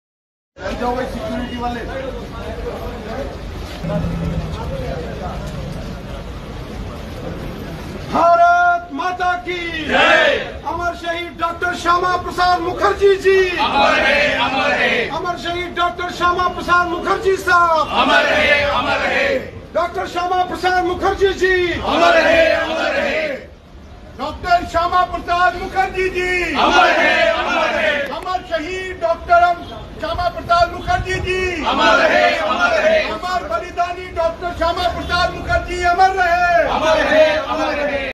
Amit Shah pays tributes to Dr. Shyama Prasad Mukherjee on his Martyrdom Day in Jammu.
Home Minister Amit Shah pays tributes to Dr. Shyama Prasad Mukherjee on his Martyrdom Day in Jammu.